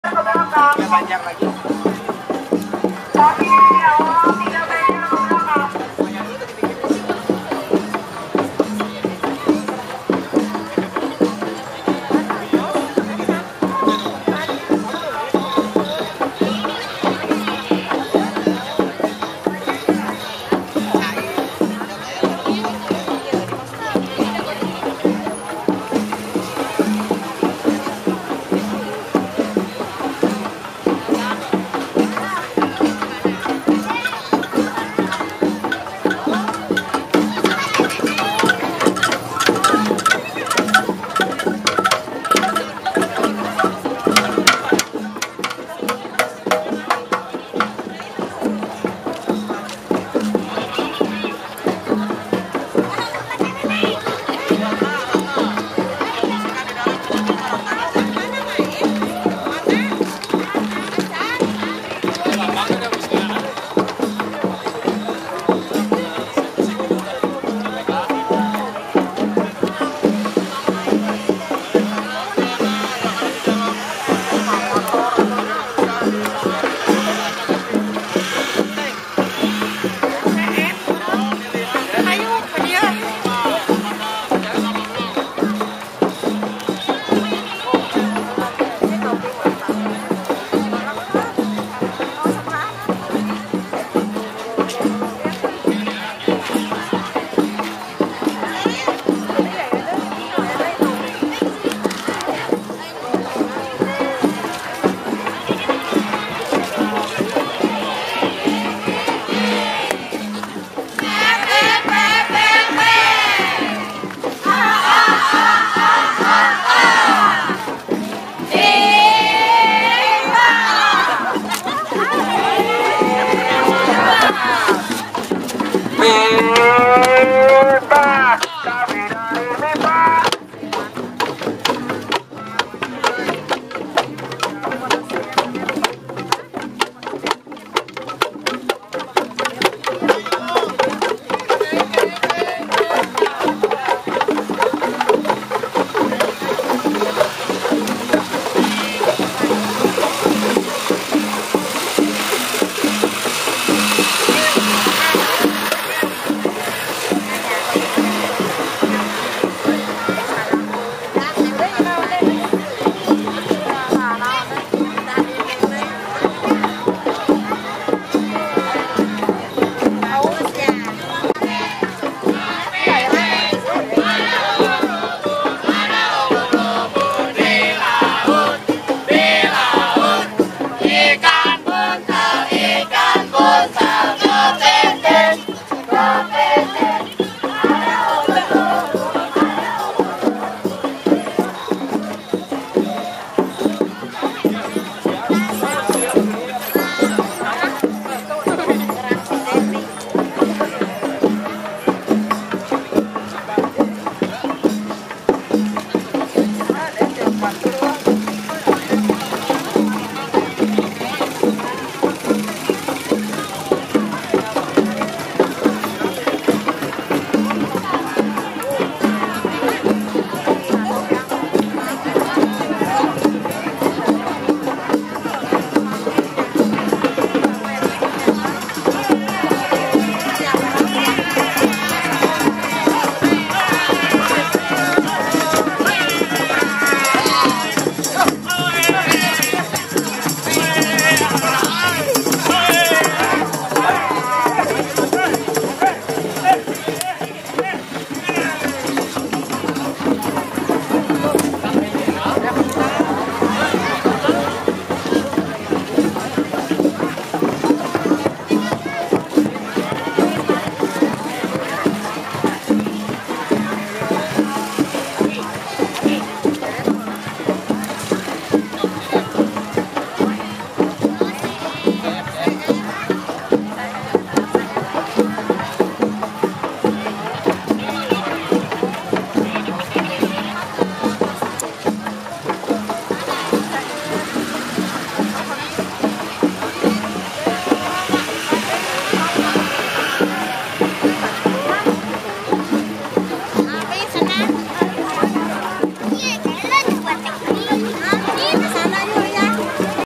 Gondang Panggeuing Rasa Ulang Tahun UNPAD Part 1 clip11
UNIVERSITAS PADJAJARAN DIES NATALIS UNPAD 59 PANITIA OOTRAD 9
Nomor Undangan : 01/EKS/OOTRAD-9/VIII/2016 Undangan dalam rangka acara Olimpiade Olahraga Tradisional (OOTRAD) yang ke-9 pada Hari Minggu 28 Agustus 2016 bertempat di Kampus Universitas Padjadjaran Jatinangor,Sumedang untuk mengisi salah satu Eksibisi permainan tradisional Sunda (Nabh Lisung atau Ngagondang)